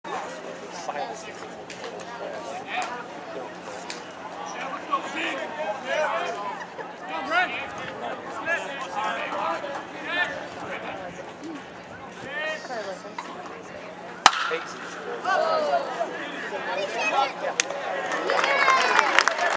Akron Jr. CF Devan Ahart 3.15.14
Akron junior outfielder Devan Ahart against Cincinnati, March 15, 2014.